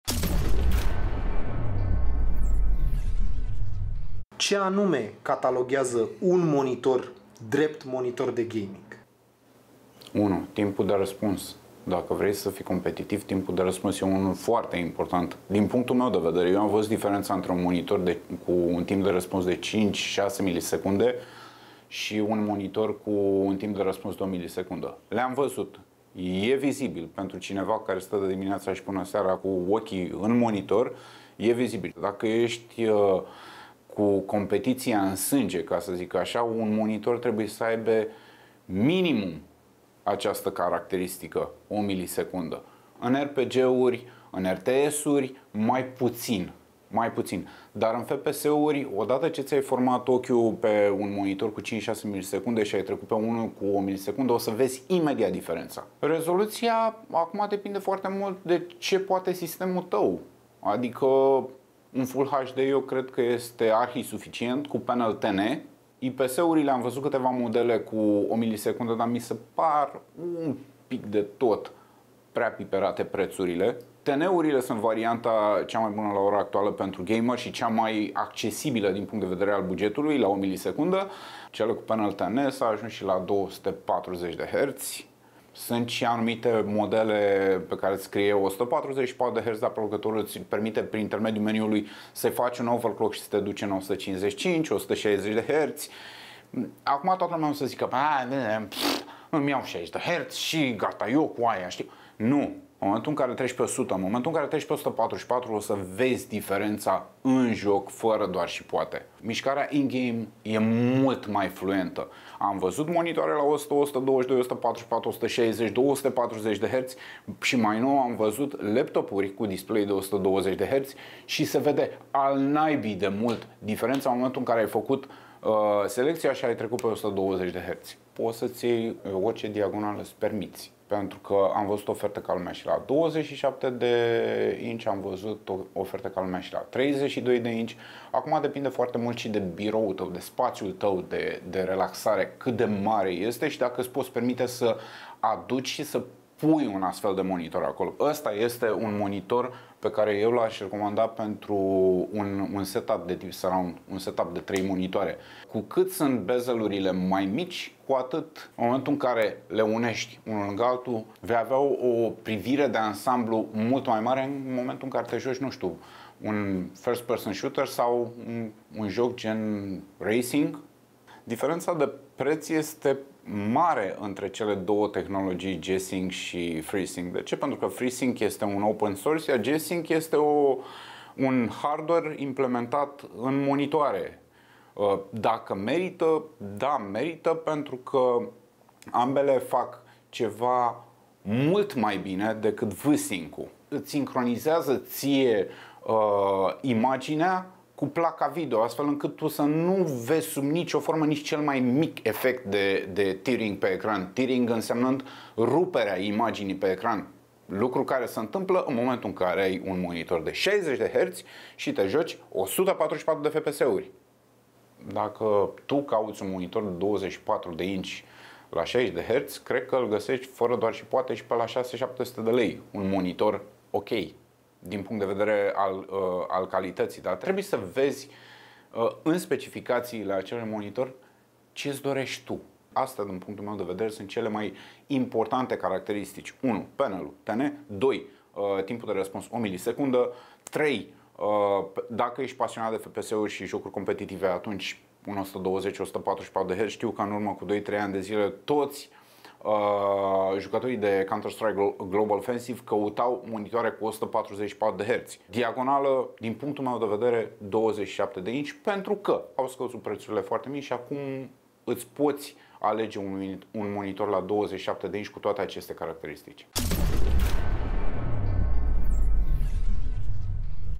PC Garage: Cum iti alegi un MONITOR BUN si IEFTIN pentru GAMING?
Care sunt criteriile cele mai importante in alegerea unui monitor de gaming?

Material realizat de ION MIHAI ANDREI